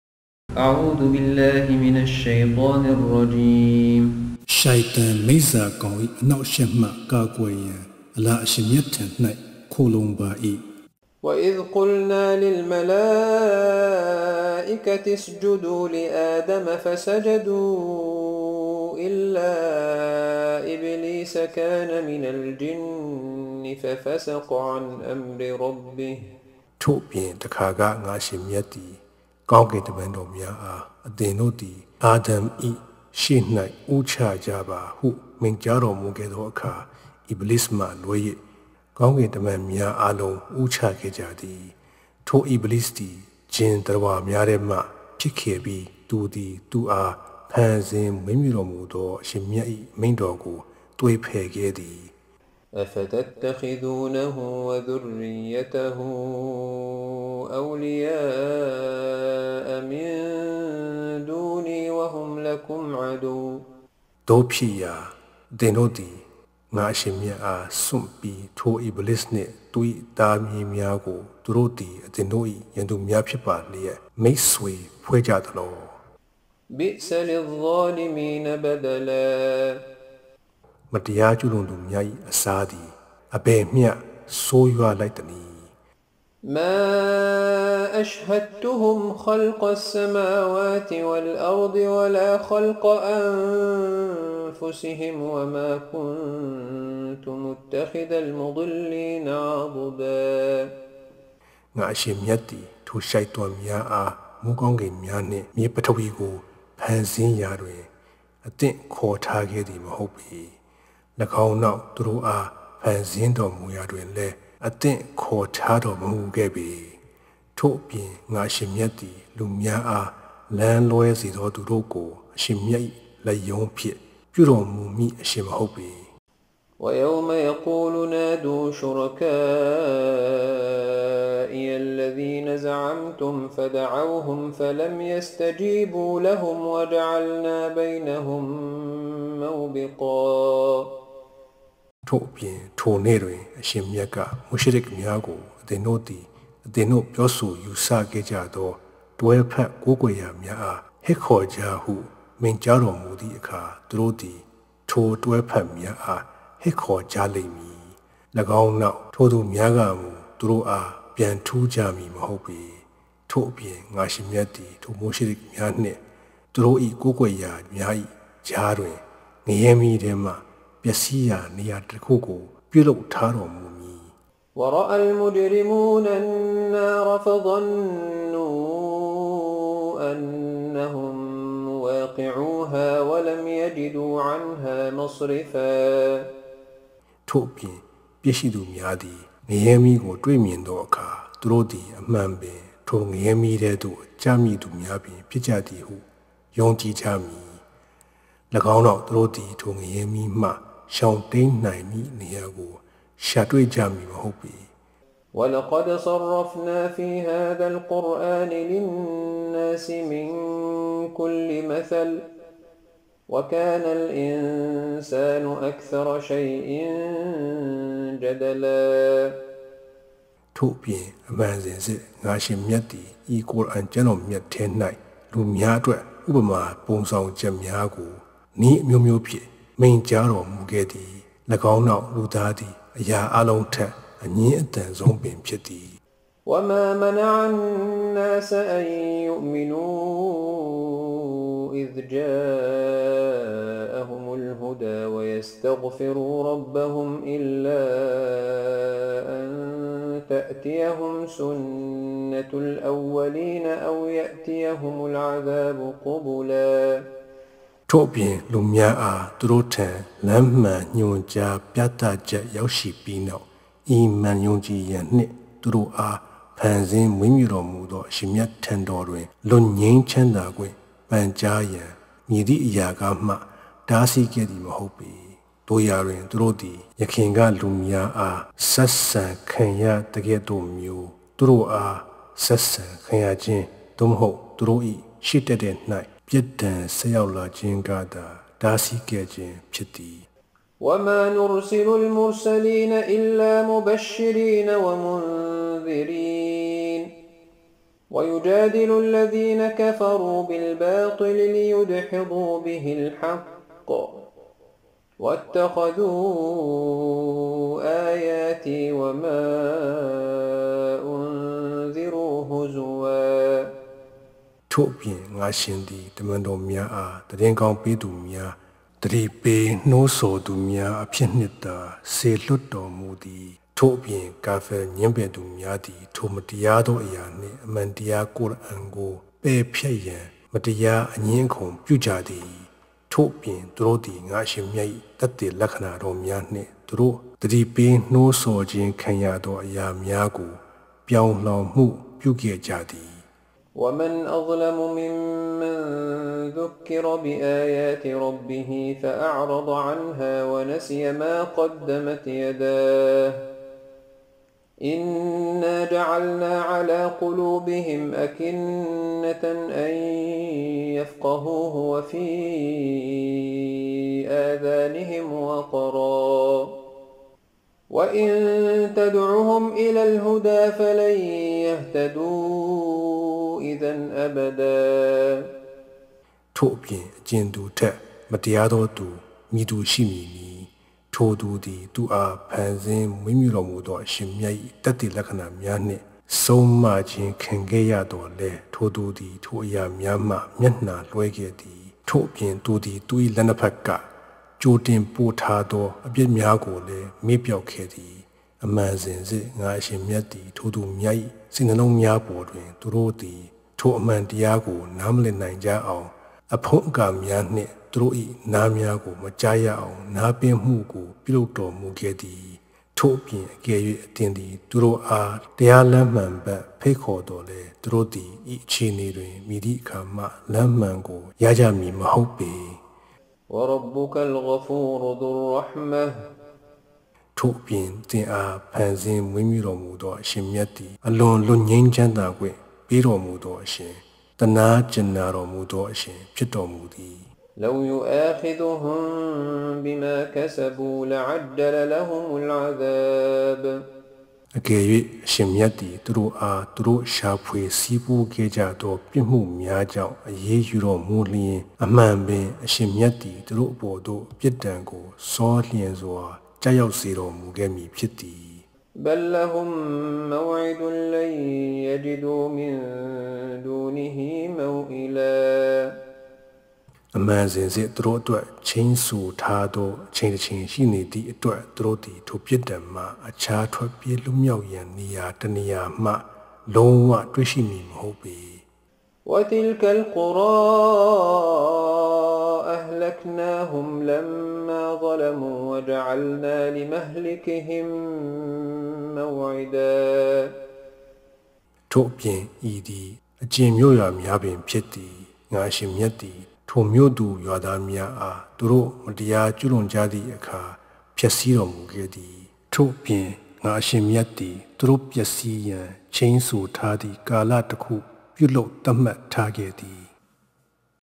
စူရဟ် ကဟ်ဖ် ( လိုဏ်ဂူ ကဏ္ဏ ) အာယသ် ၅၀ - ၅၉ - Story of Iblees (Satan) - Quran in Burmese
စူရဟ် ကဟ်ဖ် ( လိုဏ်ဂူ ကဏ္ဏ )

အာယသ် ၅၀ - ၅၉

Quran in Burmese translation recitation. Quran recitation by Dr. Sheikh Islam Korma. Burmese recitation by Siddiq.